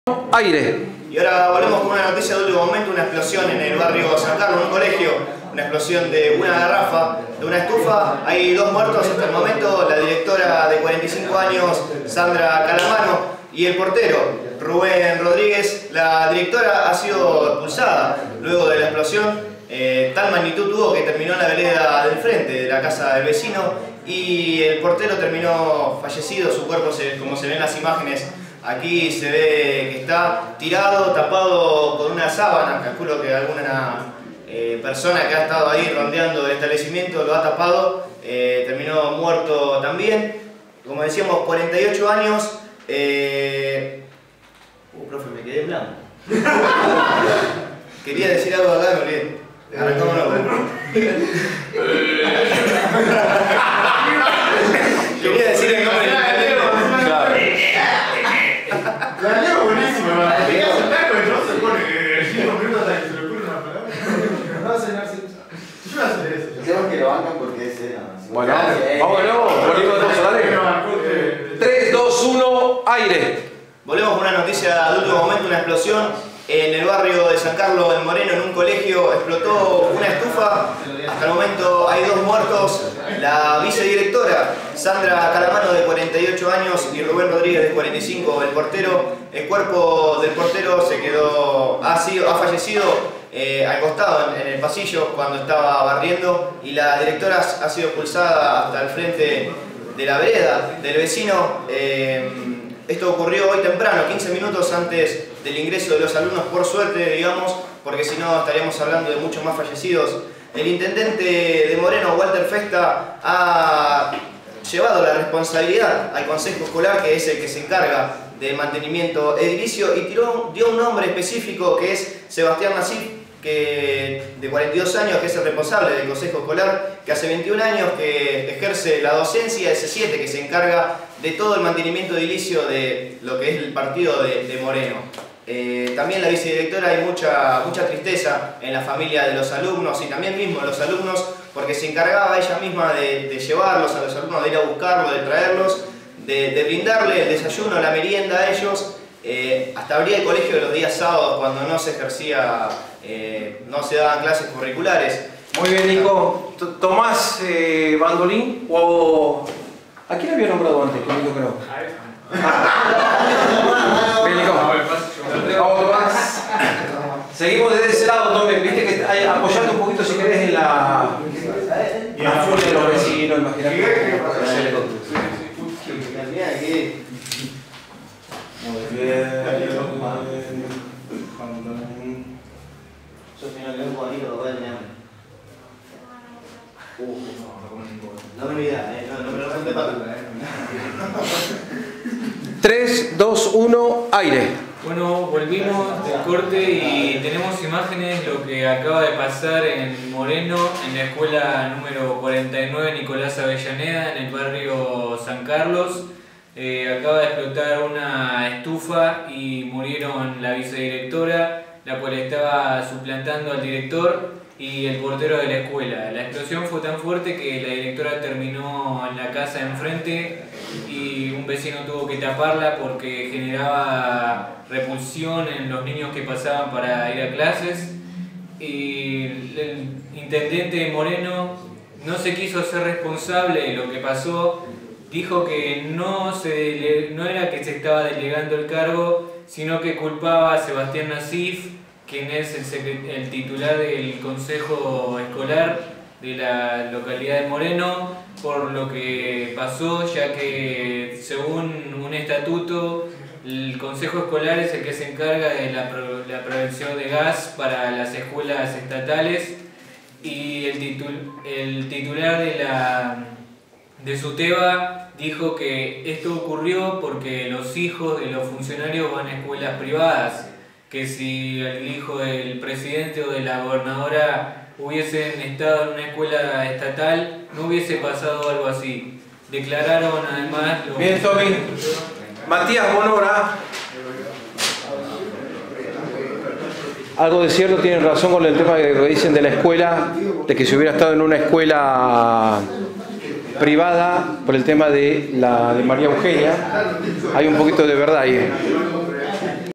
Guillermo Casas clase práctica TV I 2do B en el ISPeD. Parte XV.
Clase a cargo del periodista e integrante del Instituto Superior ISPeD Guillermo Casas. El docente, pone manos a la obra a los alumnos de 2do división B del turno noche 2018.
Participan los alumnos Víctor Nicolás Ávalos y Tomás Bandolín.
Periodismo televisivo para todos en un breve y divertido resumen.